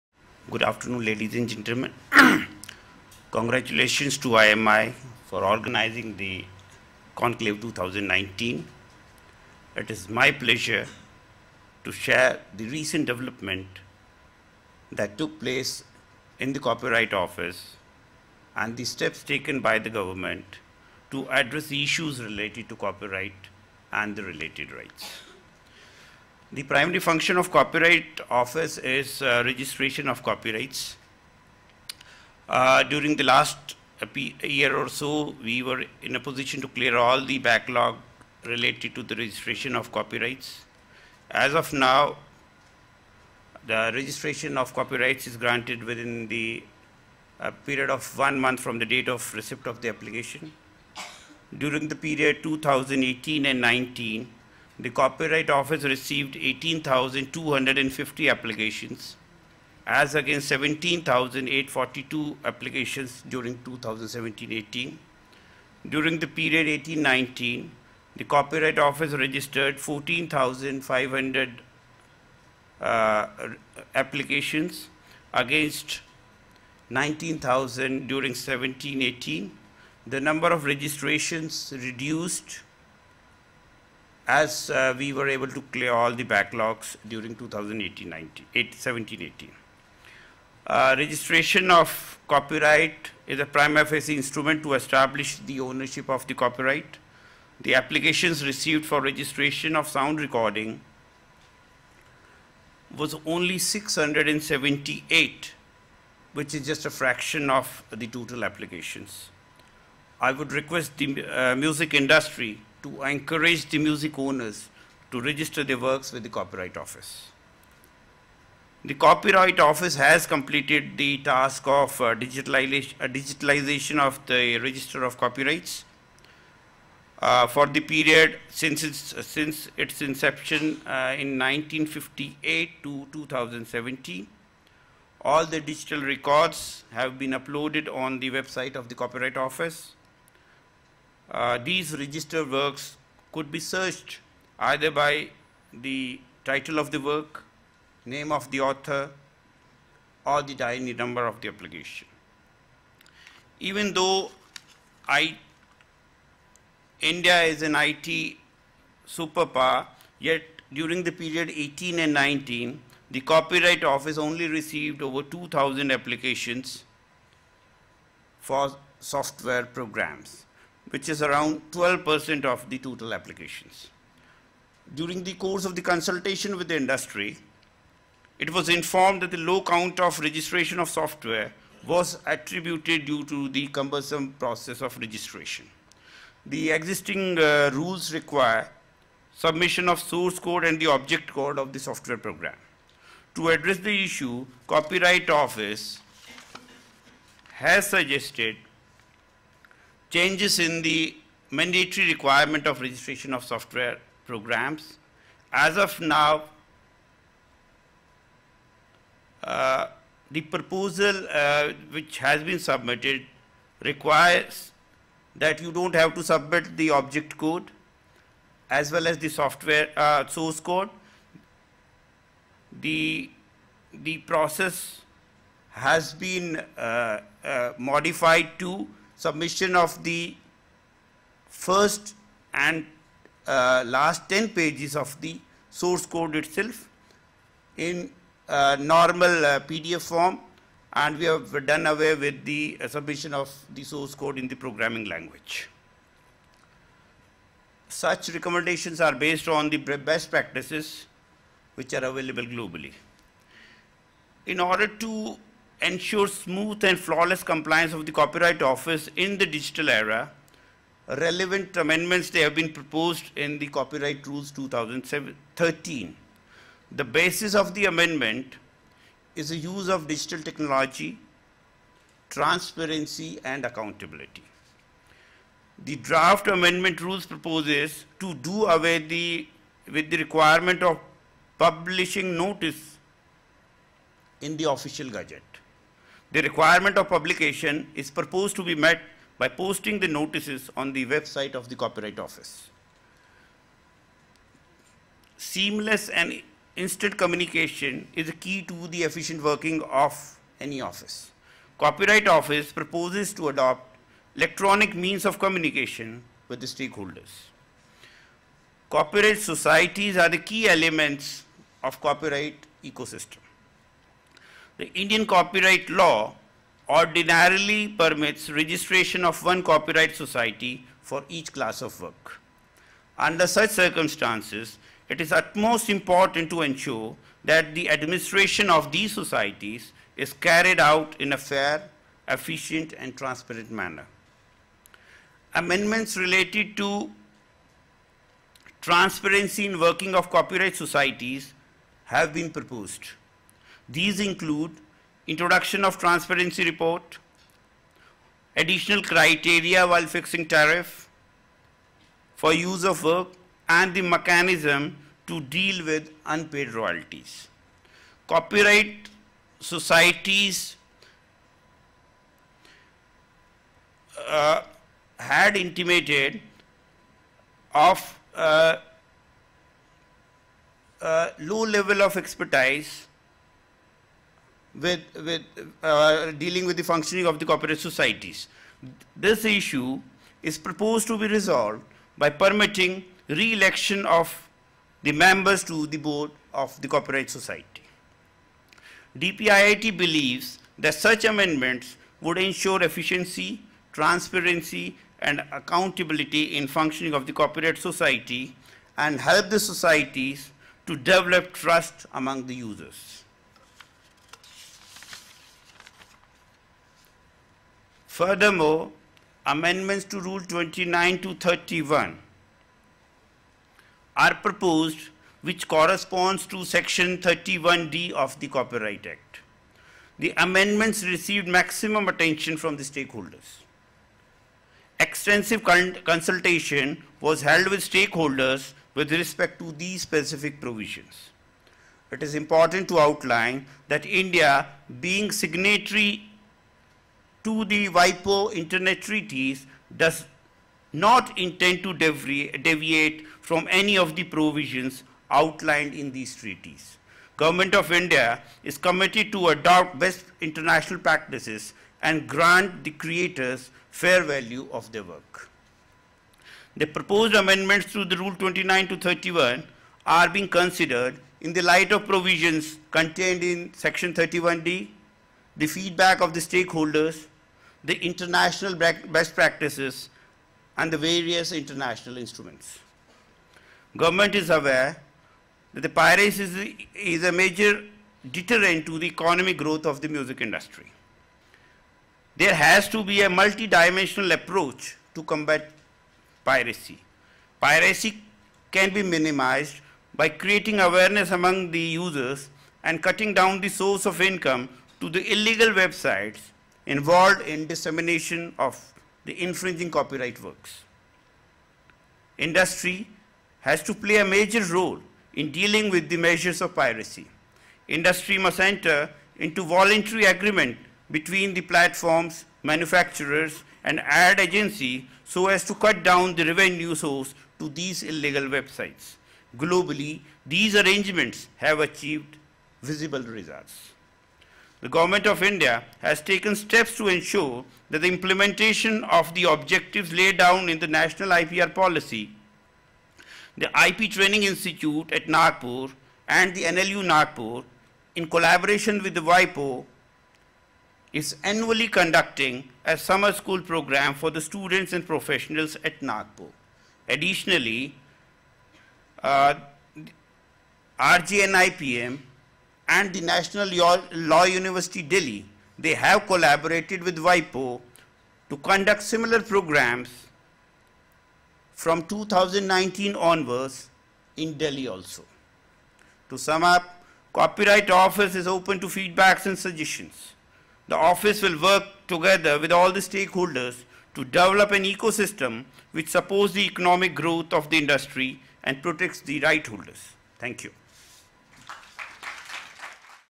Dialogue 2019: Keynote Address- India Copyright Office, Hoshiar Singh ITS, Registrar of Copyright
Keynote Address by Hoshiar Singh ITS, Registrar of Copyright, India Copyright Office.